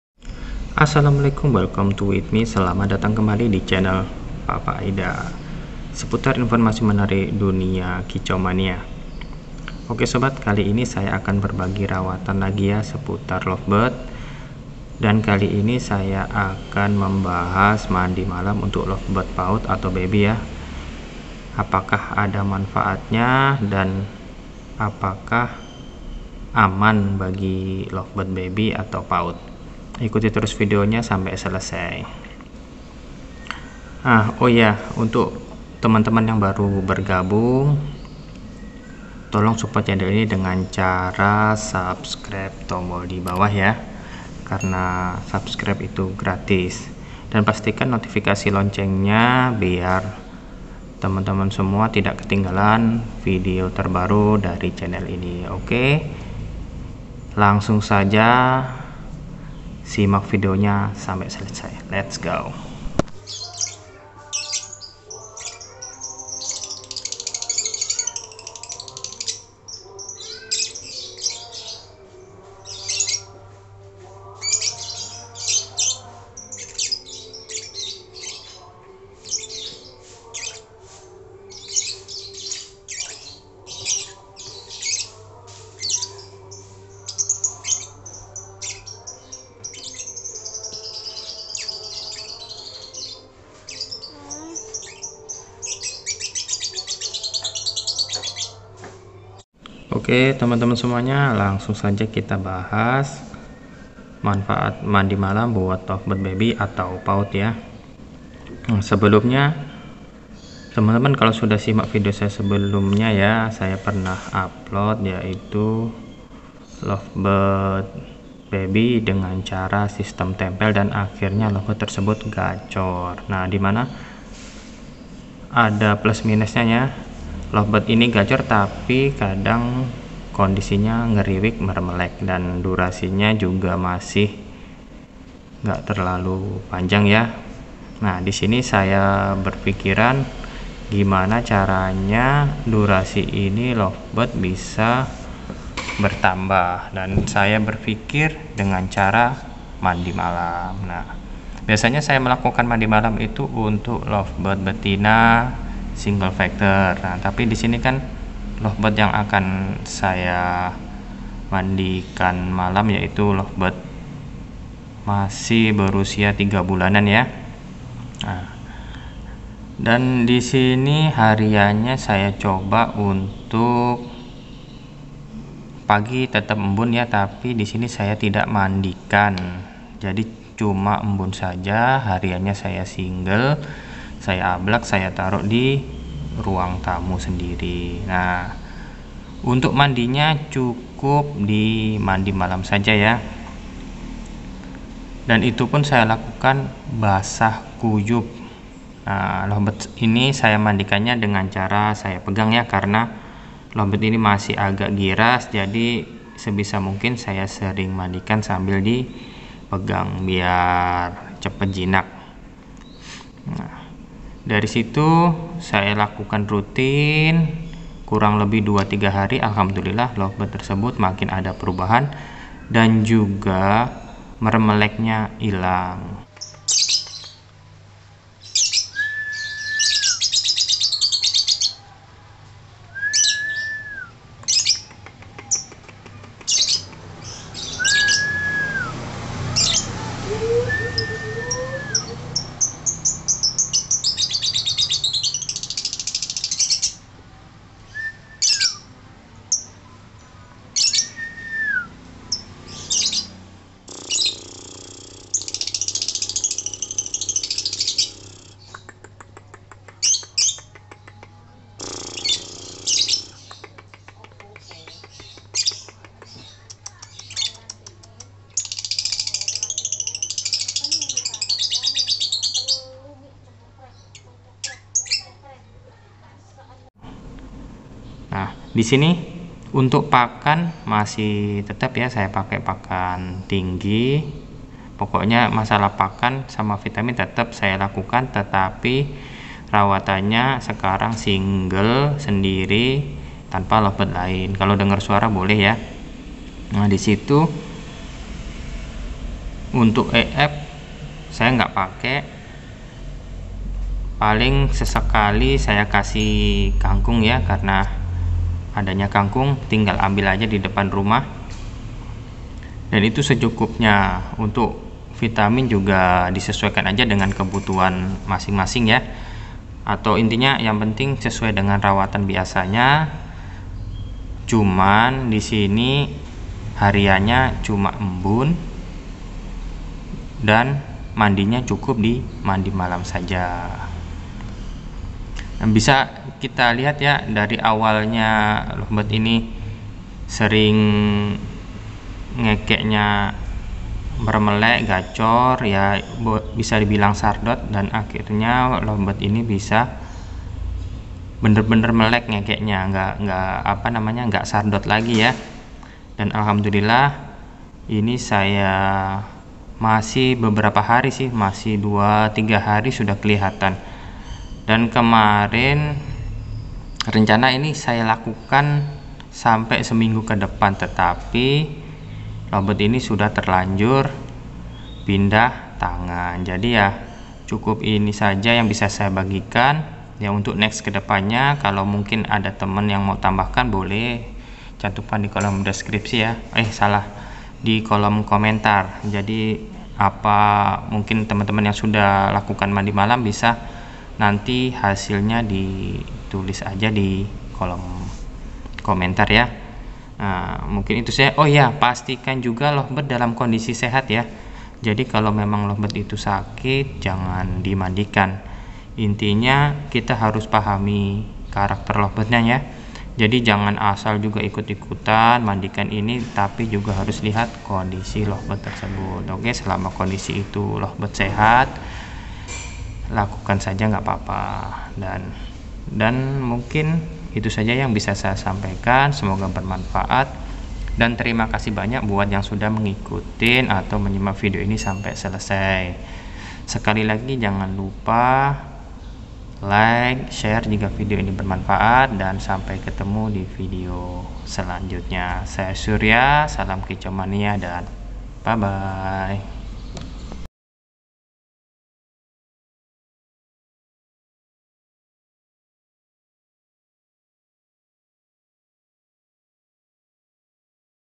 Manfaat MANDI MALAM untuk Lovebird BEBY , BALIBU / PAUD - Bongkar Durasi Lovebird
Manfaat mandi malam untuk Lovebird beby, balibu / paud. Tidak hanya untuk Lovebird dewasa tetapi manfaat bisa dirasakan juga pada lovebird Beby, balibu / paud. Untuk hasilnya simak video sampai selesai ya. Terimakasih 🙏.

Record with Xiaomi Mi A2
Video Editing with Kinemaster